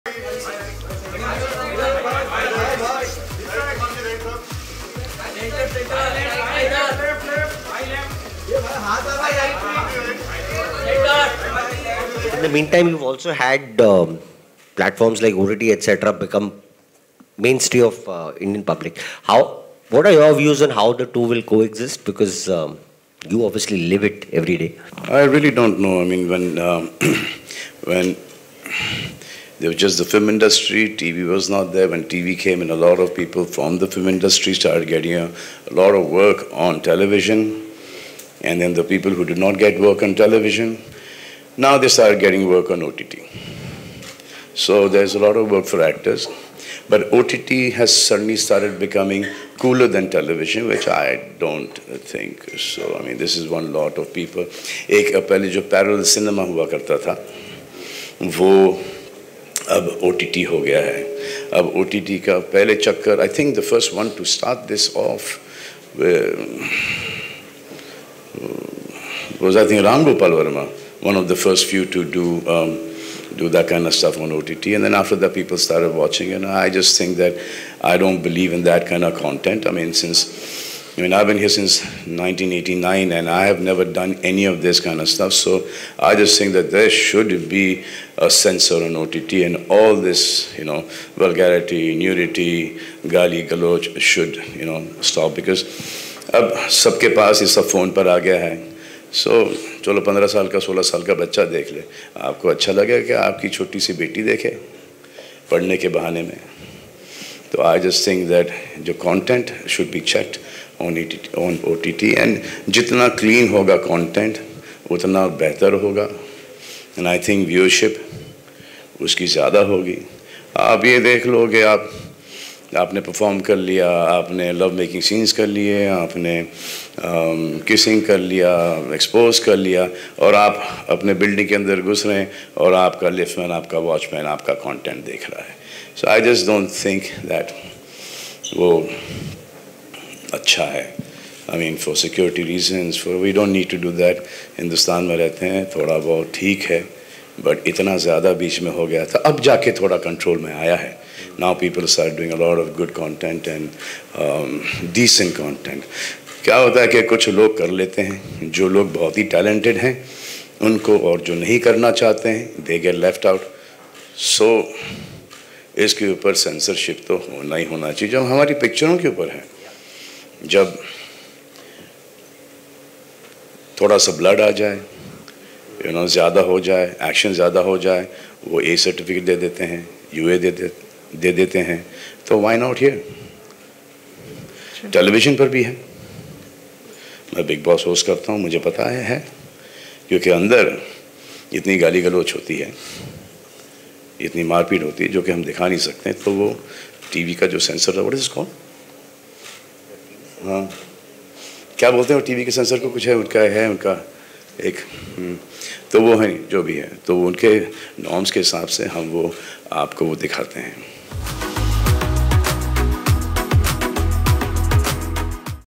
Salman Khan Talks on Censorship on OTT | ‘Vulgarity, Nudity, Abuse need to Stop, We Live in India’
Salman Khan expressed concern about the lack of censorship on OTT platforms and emphasized the need for cleaner content for the younger generation. Watch Entertainment Videos.